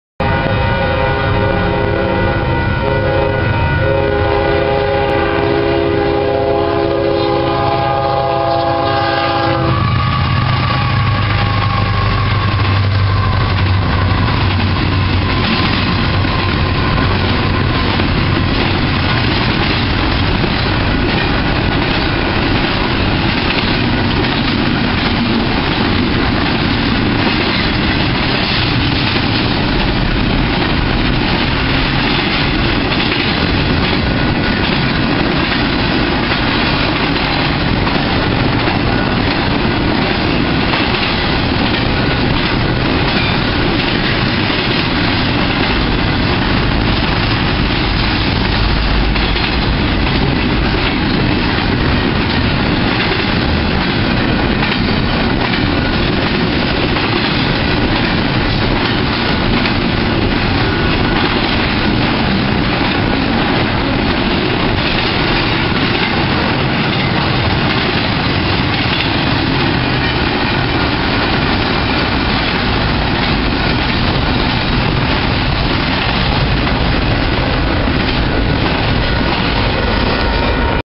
Arlington, Texas - UP 5846 - 09/29/2010
UP 5846 leads an eastbound intermodal thru town.